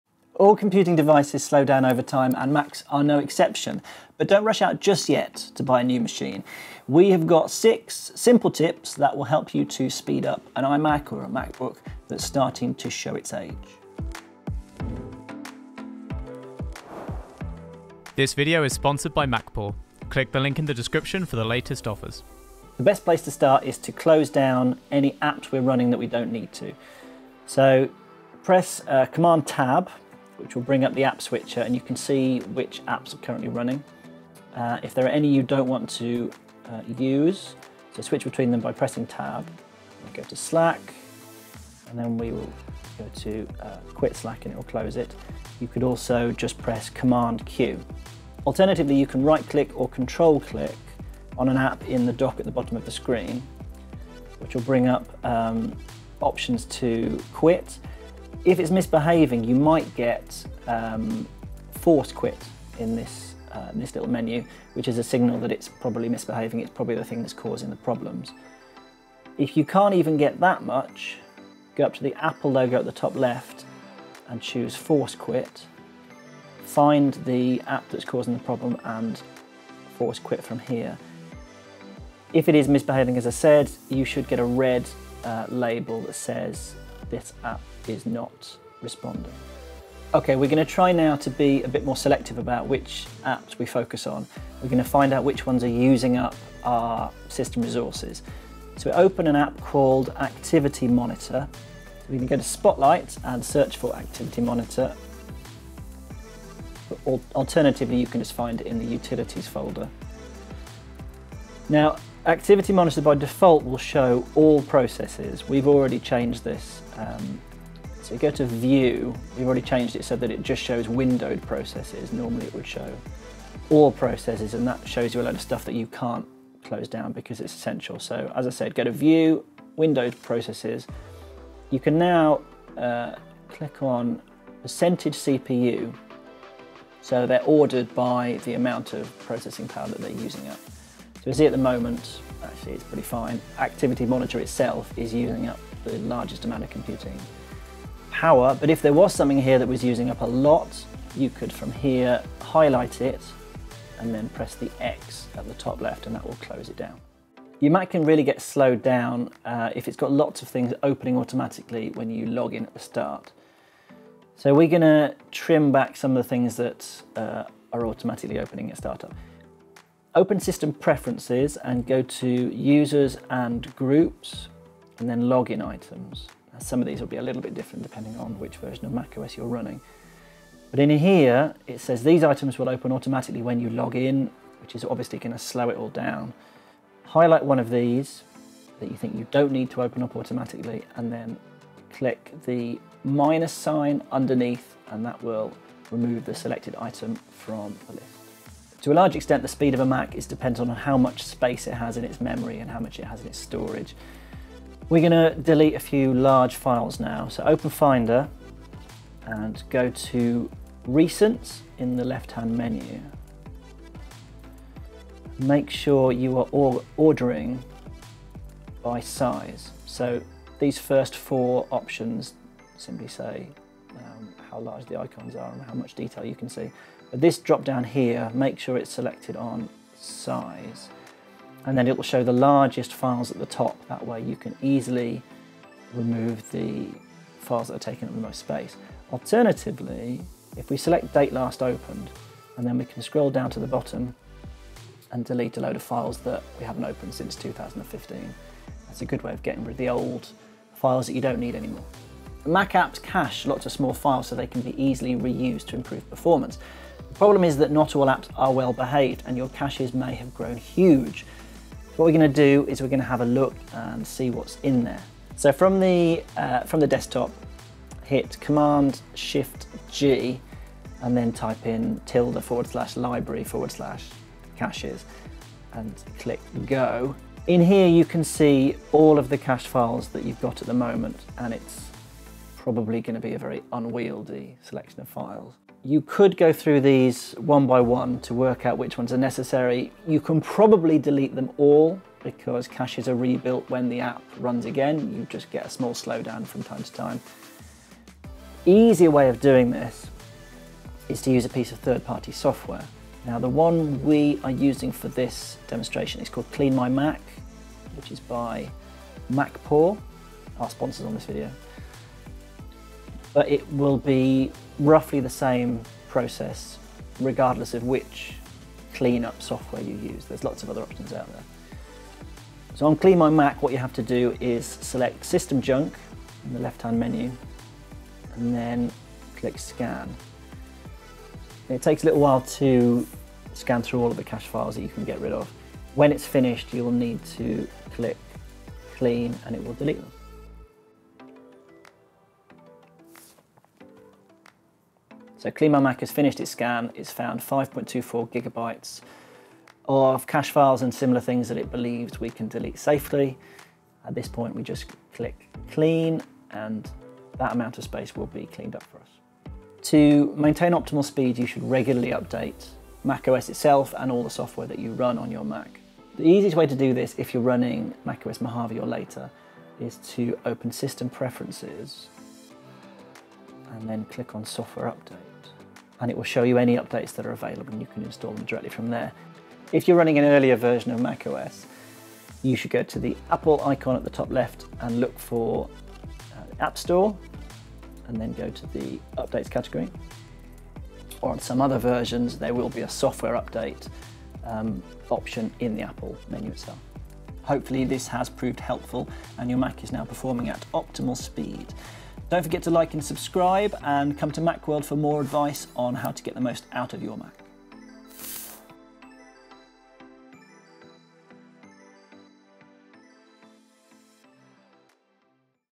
6 simple tips to speed up your Mac
In this video David shows us how to speed up a Mac with six simple tips.

Shot on a Canon C100 Mark 2

Follow Macworld for more iPhone and Apple Updates!